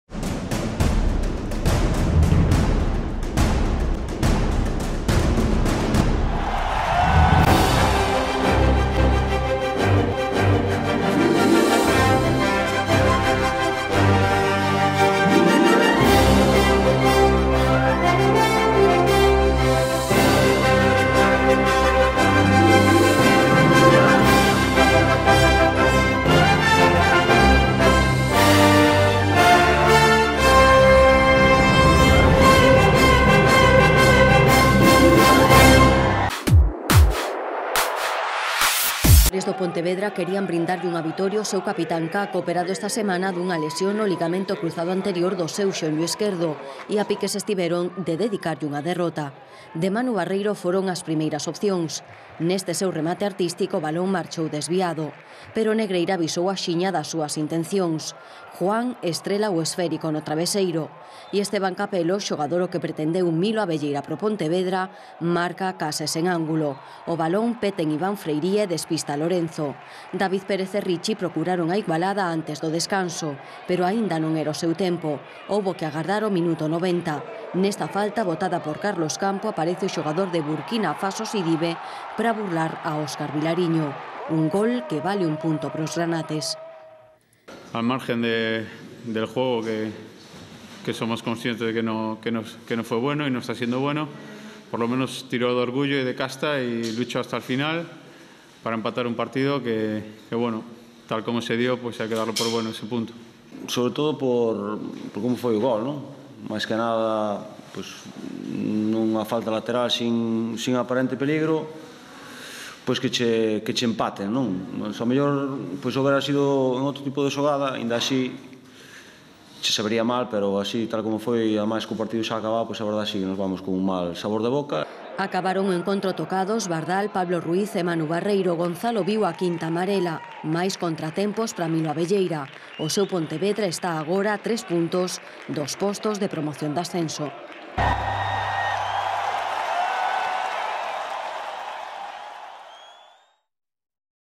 Pontevedra CF- Negreira CF Declaraciones jugadores y entrenador
Al termino del partido los entrenadores y jugadores nos dan su opinión sobre lo acontecido en el terreno de juego.

Resumen gracias a TVG2